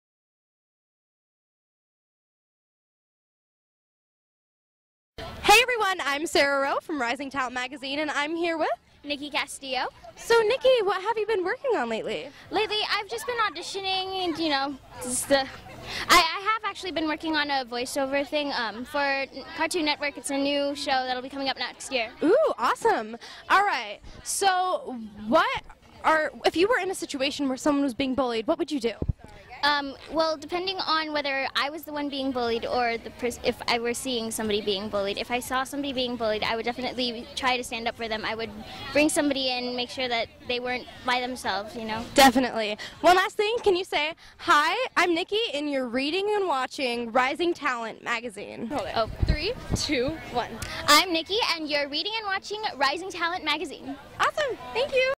RTM Update Interview NIKKI CASTILLO
On Saturday, April 22nd 2017 while attending The Say No Bullying Festival, Rising Talent Magazine's Junior Reporter Sara Rowe interviewed NIKKI CASTILLO about what she has been up to with her acting career!